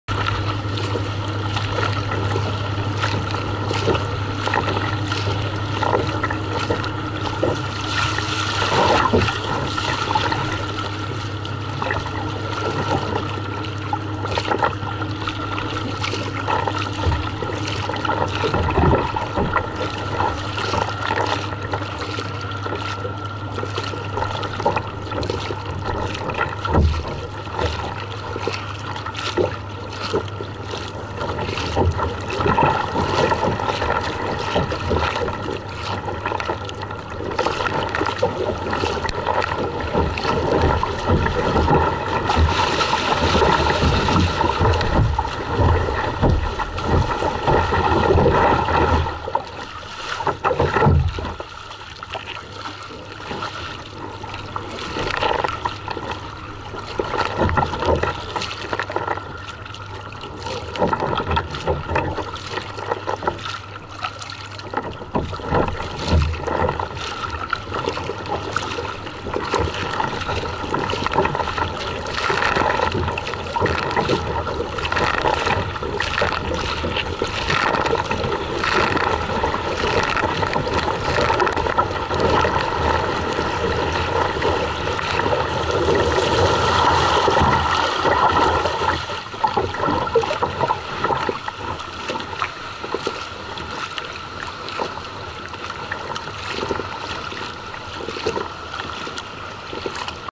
Lyckad M/S Prins Carl Philip - 3 augusti 2019
Sådär! Det tog några veckor att justera marginalerna för ny båt och relativt ny paddel, men nu är det gjort...och det var det värt! Nu börjar det bli riktigt roligt med 550:an och Jantexpaddeln:)!